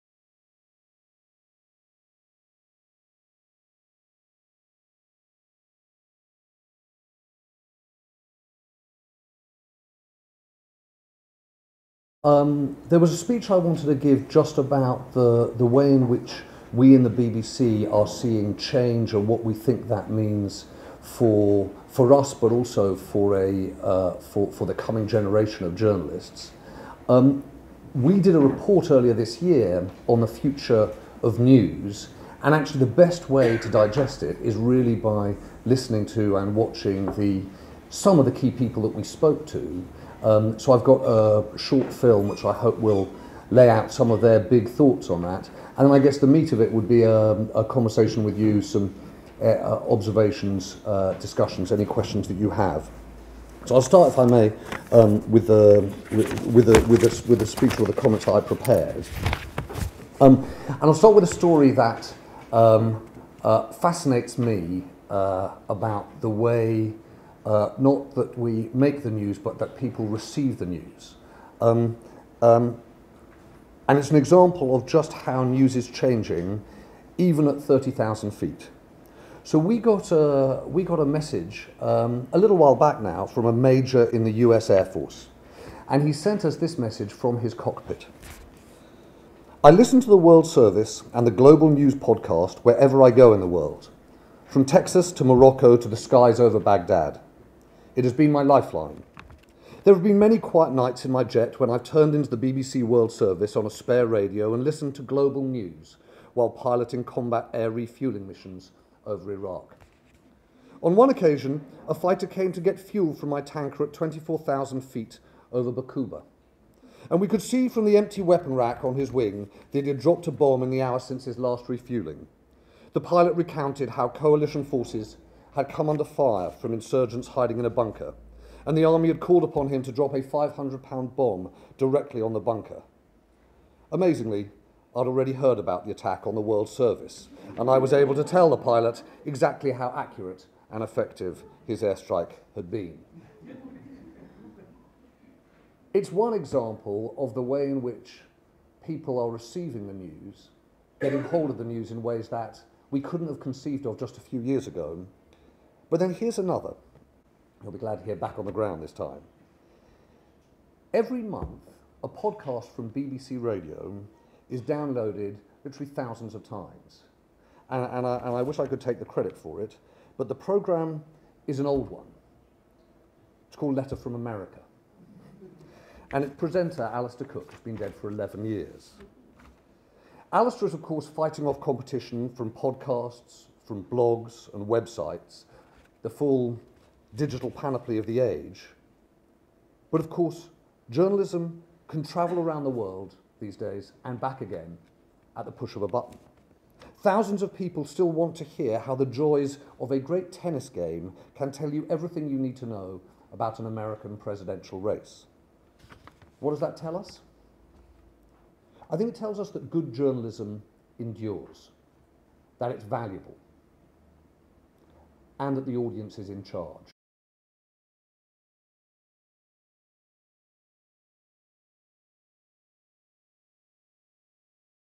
James Harding at AJE AJEFutureEd (Part 1)
James Harding, Director, BBC News and Current Affairs.

AJE - JUNE 2015 CONFERENCE

Cristiana Ferrauti and Alex Xi Zhang

Edited:
Steve Hill